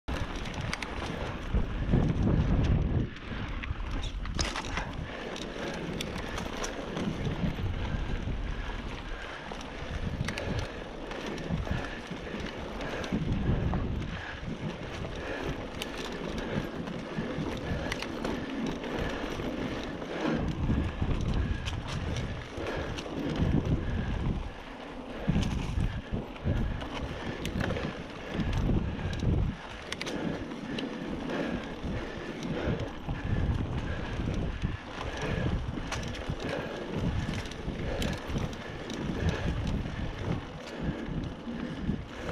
Following Pat through Lower Bidwell Park 09-2019
Following Pat through a section of Lower Bidwell Park single track.  POV from a 2016 Giant Trance 2.  Chico, CA, Sept 2019